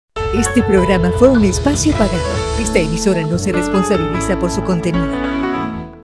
WPRO-TV 8 Guayama, PR - Fuegito O&O - Paid Programming Advisor (8/6/24 - 3:29AM)
WPRO debuted this bumper on July 29, 2024. It still uses it to this day.